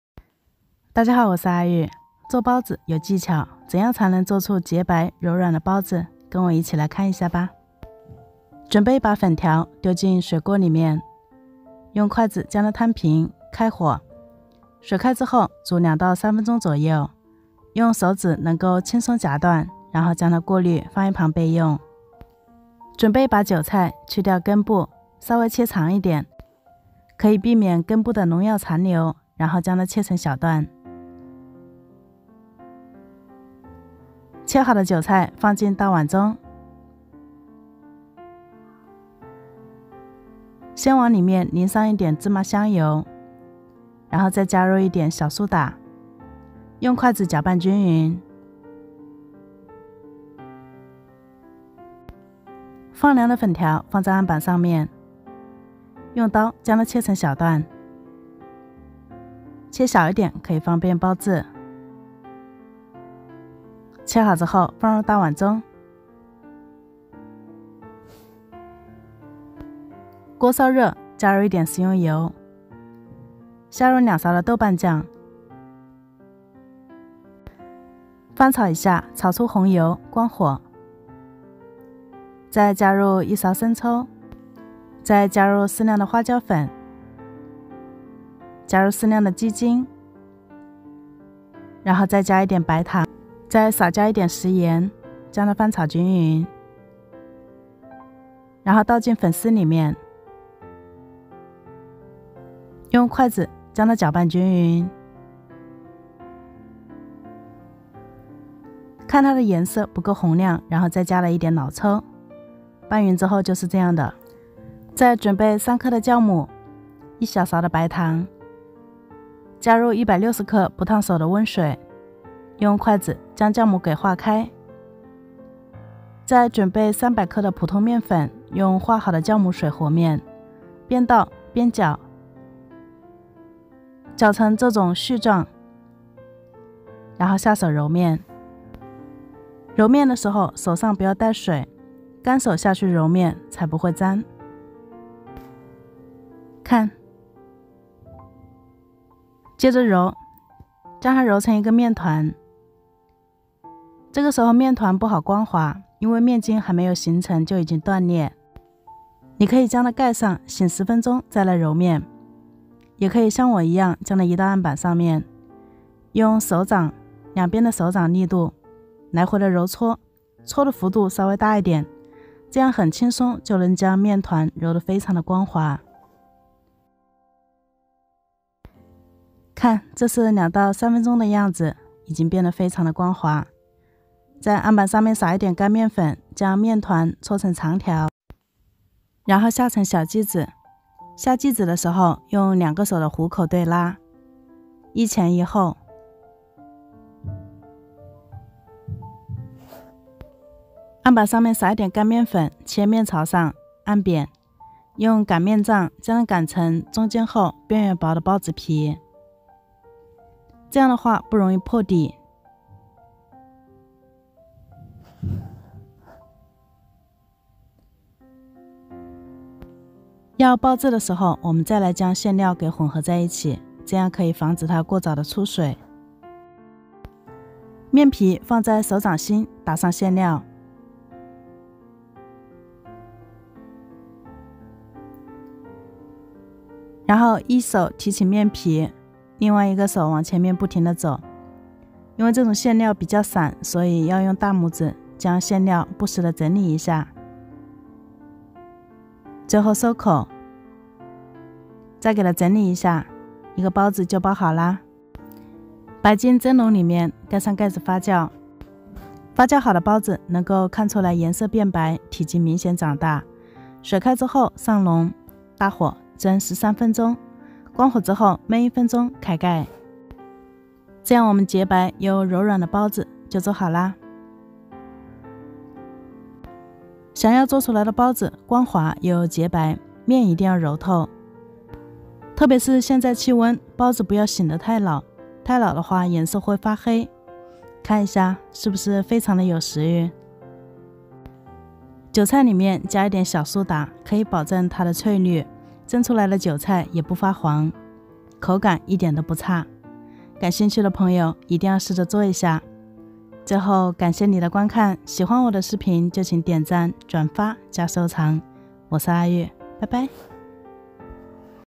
要想包子松软又洁白，揉面是关键，做好的包子蓬松不塌陷
每天都會為你更新一道中國#美食 家常菜 面食 小吃 的做法

阿玉愛美食，謝謝您的喜歡